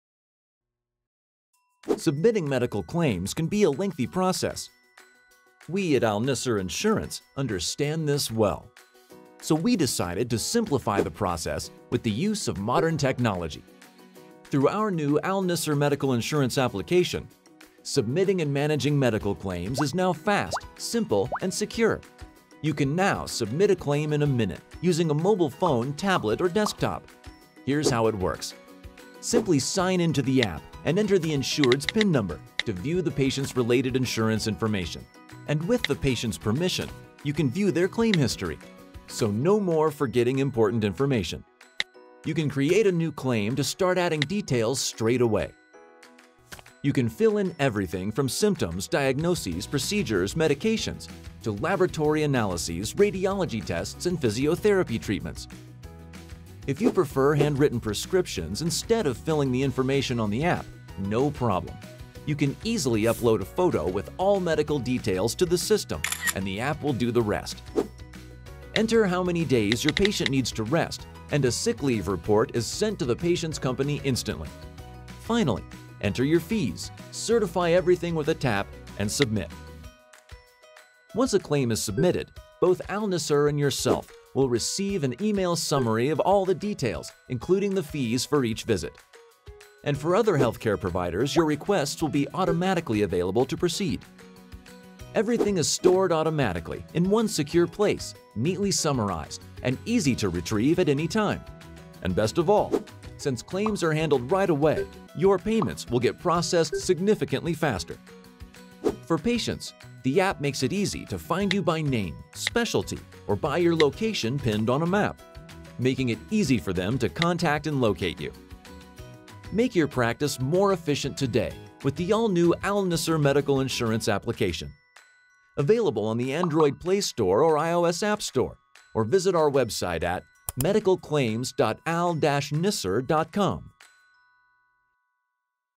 Al Nisr Al Arabi Insurance Medical App
Al-Nisr Al-Arabi Insurance Company is proud to launch a new and revolutionary mobile application and desktop software to its customers and medical network of providers, instead of the traditional paper insurance forms.
Our new app offers you as a customer an easy and seamless way to manage your medical profile with a tap of a button. Once you log in you will be able to access your personal insurance information, view number of available medical eforms, keep a record of your doctor’s visits, access prescriptions' details, search through a wide range of healthcare providers and many more.
We’ve also introduced our web/mobile solution to the medical healthcare providers in our network. The application allows doctors to add the prescription to your profile where it will instantly appear to the pharmacies or labs when you visit to claim your medicine or take your prescribed test. Making sure your privacy is protected in each and every step in the process where healthcare providers and doctors fill authorizations based on their type/specialty and only access sections related to their role.

At this stage, the app has been rolled out and activated for a certain number of our medical insurance customers. Going forward, more organizations and companies will be added and contacted by a dedicated team from Al Nisr Al Arabi to support with the launch.